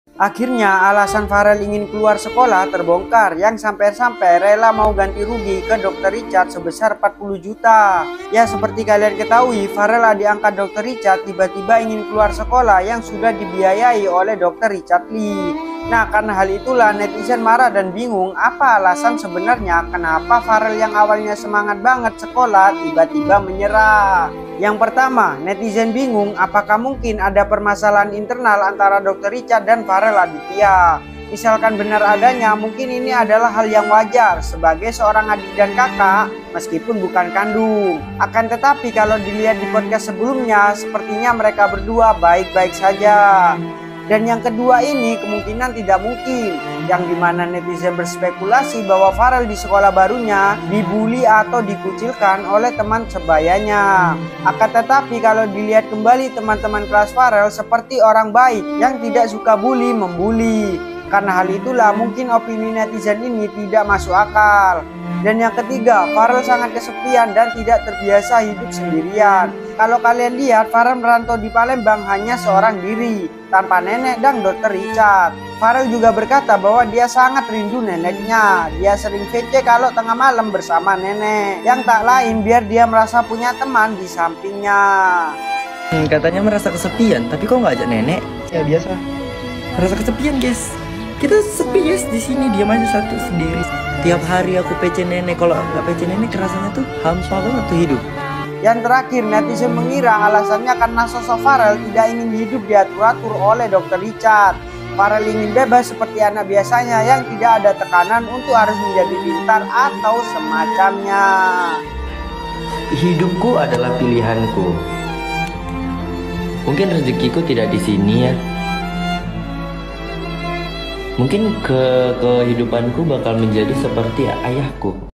ALASAN FAREL ADITYA MAU KELUAR SEKOLAH!!! FAREL DIMINTA GANTI RUGI 40 JT DR RICHARD
ALASAN FAREL ADITYA MAU KELUAR SEKOLAH YG DISURUH GANTI RUGI 40 JUTA DR RICHARD LEE